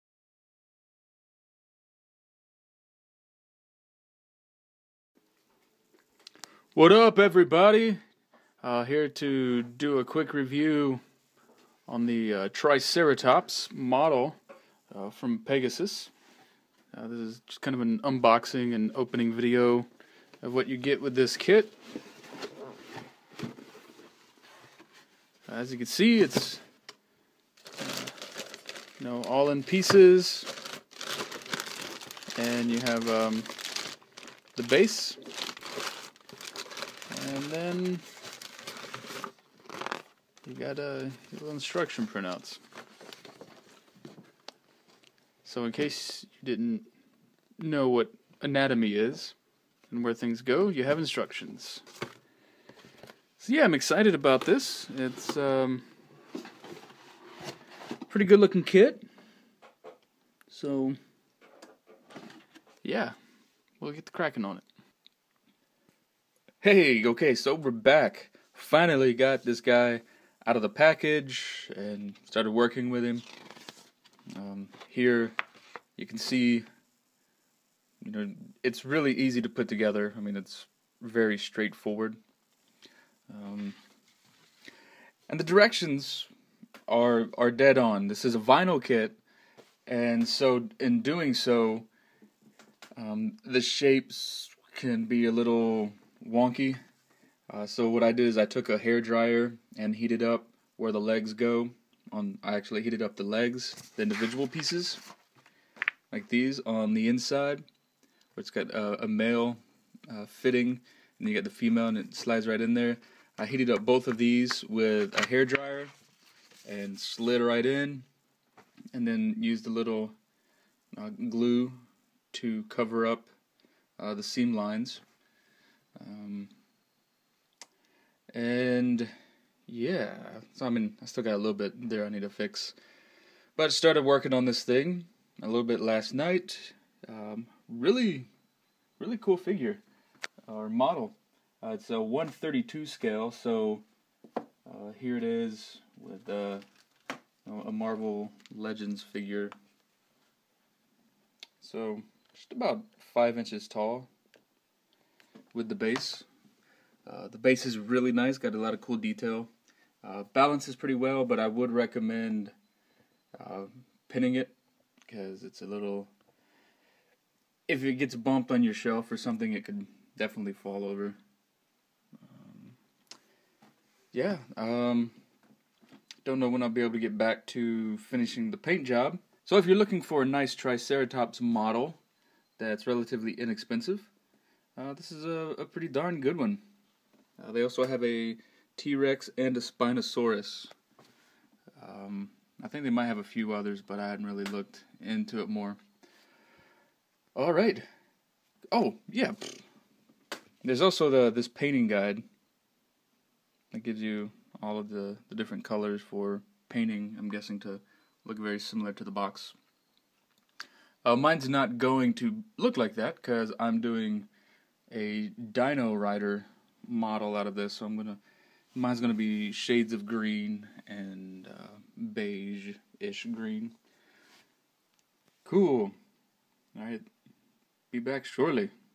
Pegasus Hobbies Triceratops Dinosaur 1/24 Model Kit
This is a very nicly sculpted Triceratops model From Pegasus. It is vinyl so I suggest heating it up before the build.